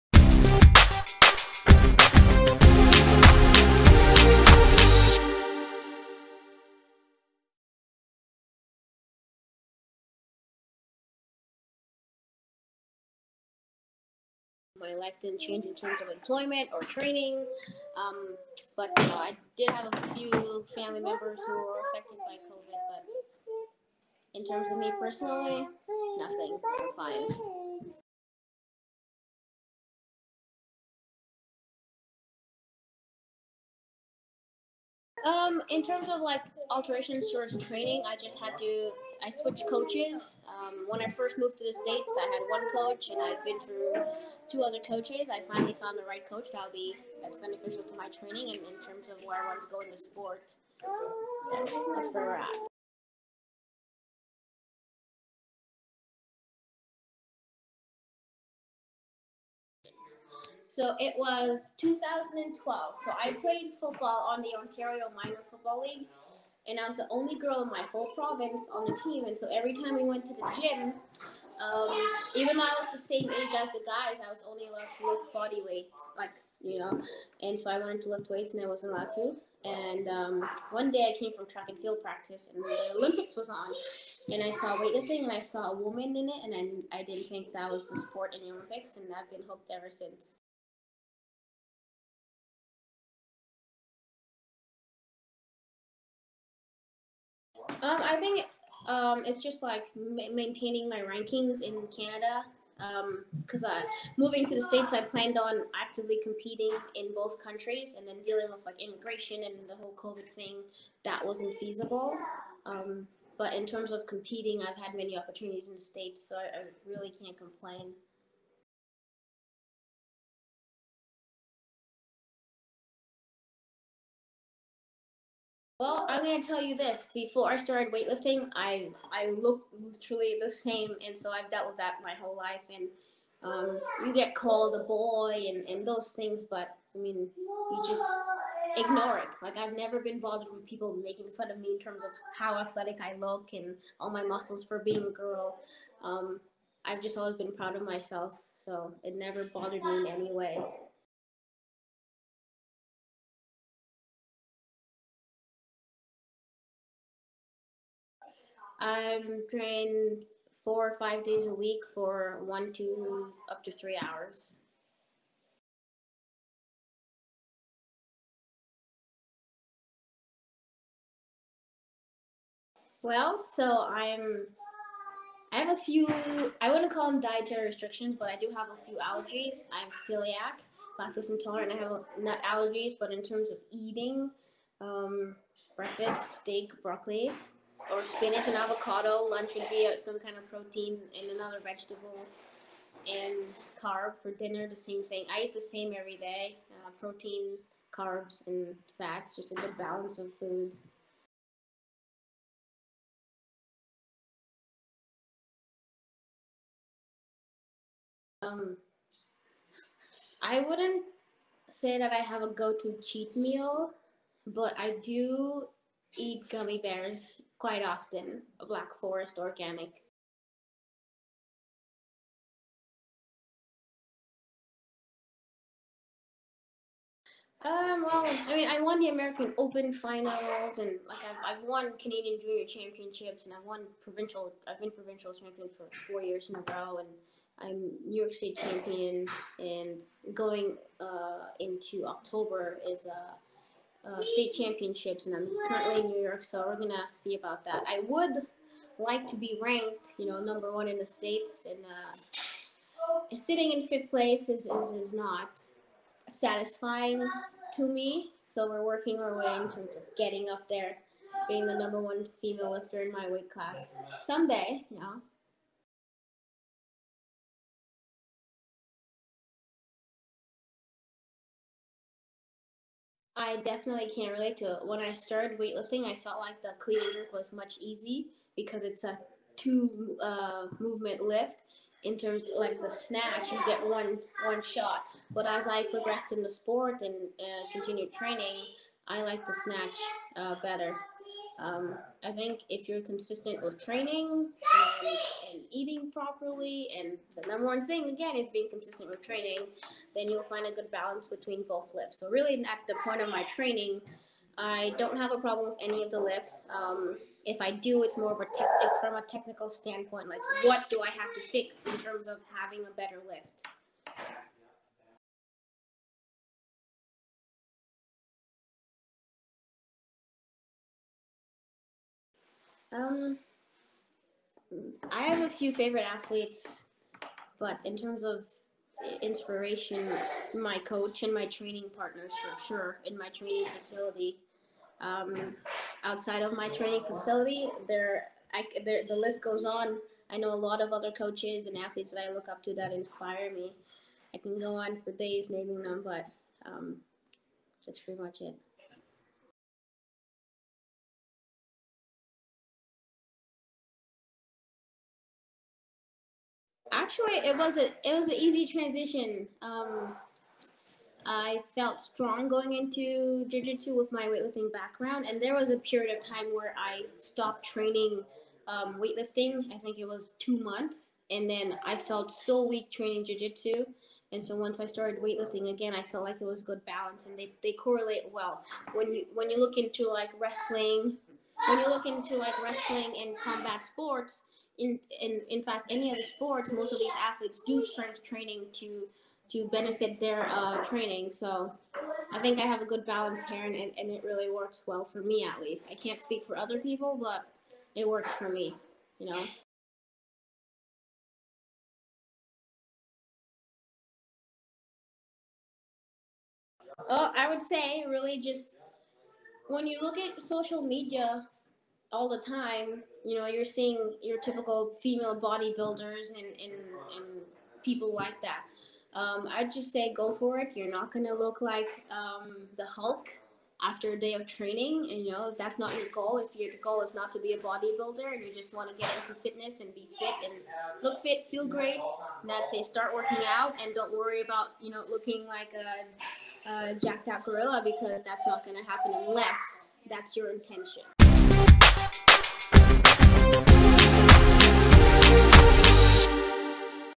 FemFit Podcast Ep. 17 - Jessica Jean-Augustine
Jessica Jean-Augustine is an Olympic weightlifter from Canada who currently resides in the United States. She discusses her journey into weightlifting, jiujitsu, goals and more...